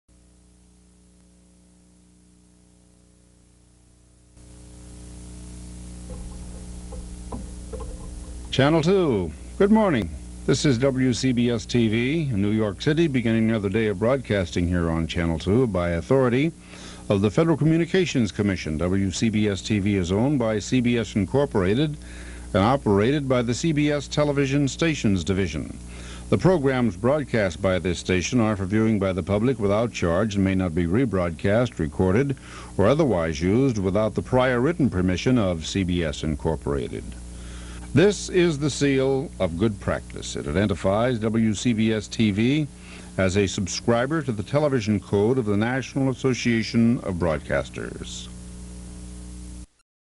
WCBS sign-on 1977
WCBS begins its broadcast day in 1977